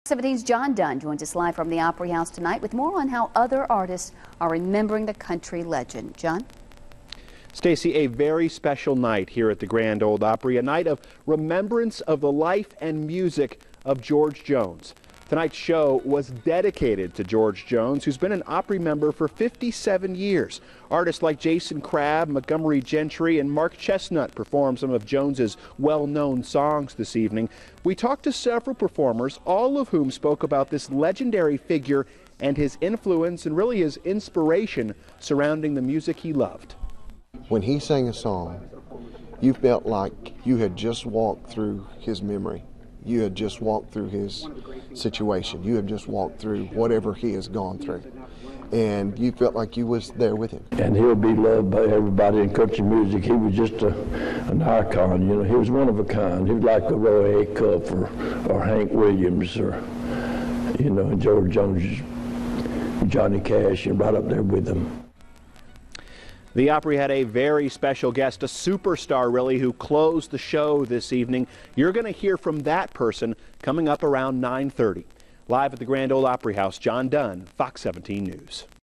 Opry Dedicates Show to George Jones
The Grand Ole Opry dedicated its Friday night show to George Jones.
Several performers sang songs made famous by Jones, who died on Friday at the age of 81.
Montgomery Gentry, Jason Crabb, and Mel Tillis were just a few of the artists who took the stage. The highlight of the evening was a surprise performance from Brad Paisley.  Paisley closed the show with a medley of Jones' songs.
Many artits talked about the enormous influence that George Jones has had on country music.